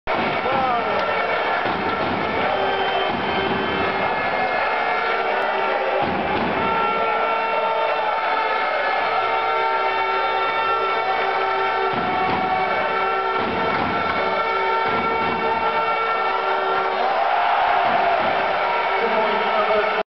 Pau Gasol scores at Eurobasket semi-final
Eurobasket semi-final, Spain vs Greece.